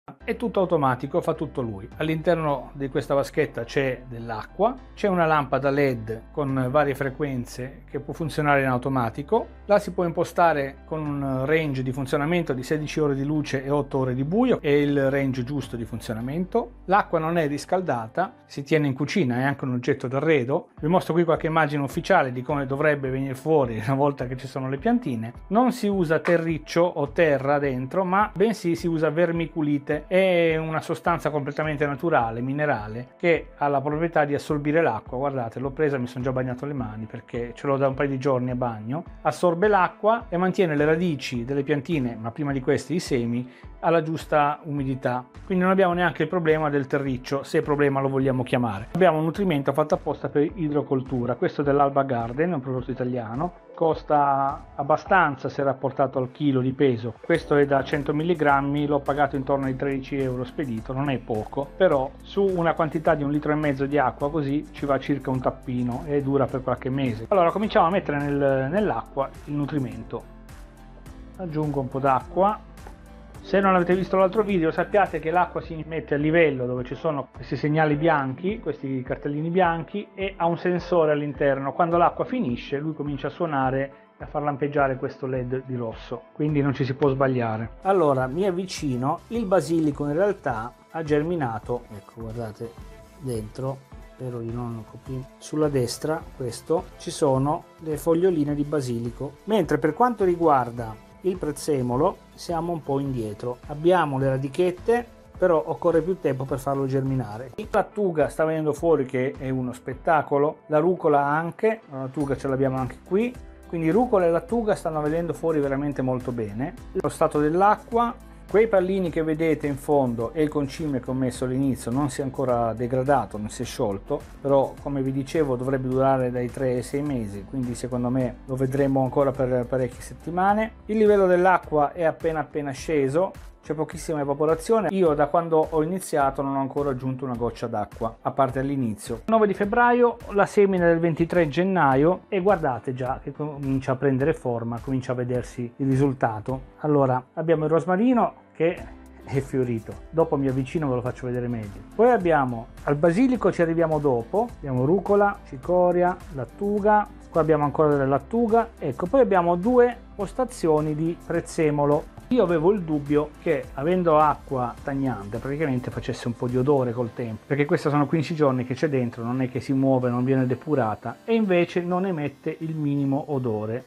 Clizia Smart Garden! Piantine di: lattuga, rucola, basilico e prezzemolo in soli 15 GIORNI!
*Dove acquistarlo

Vi mostro come in soli 15 giorni, senza alcun lavoro da parte mia, ho ottenuto le prime piantine in casa di: lattuga, rucola, basilico e prezzemolo oltre a due talee di rosmarino! Ha fatto tutto il Clizia Smart Garden!